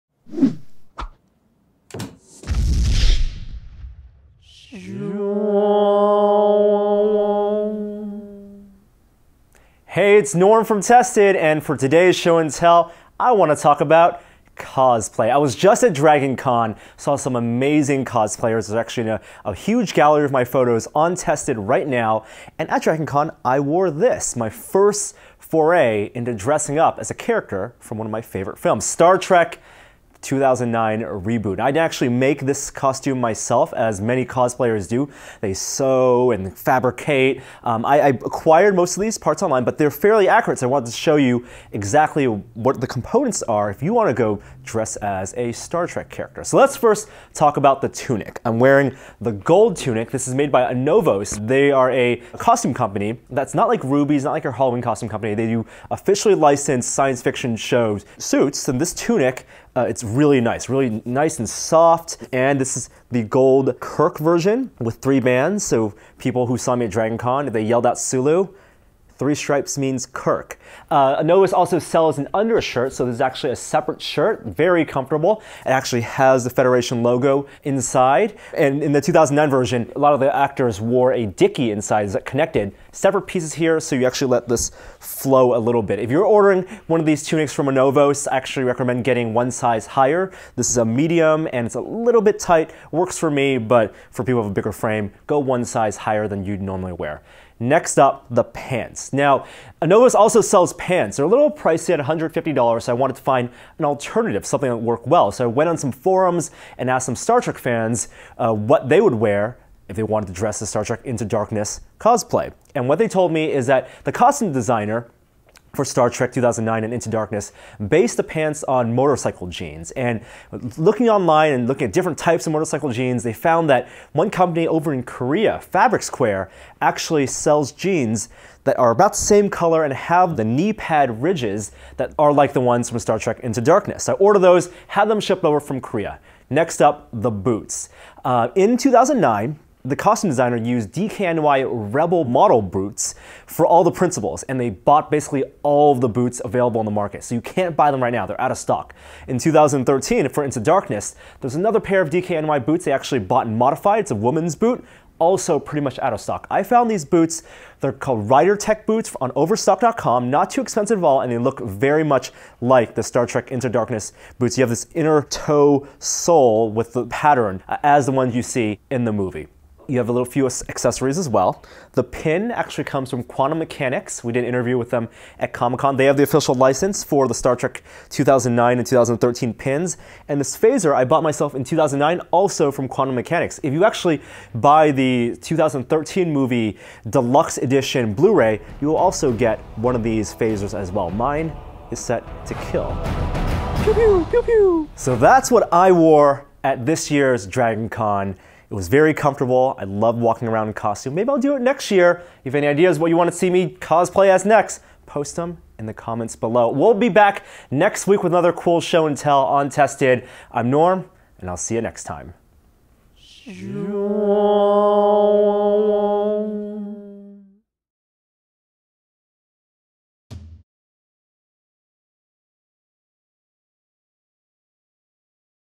Show and Tell: More Kirk Than Sulu
To take part of the festivities at Dragon*Con, Norm chose to dress up as well as one of his favorite science fiction characters, and turned to licensed costume replica companies and fan forums for tips on how to assemble an appropriate outfit.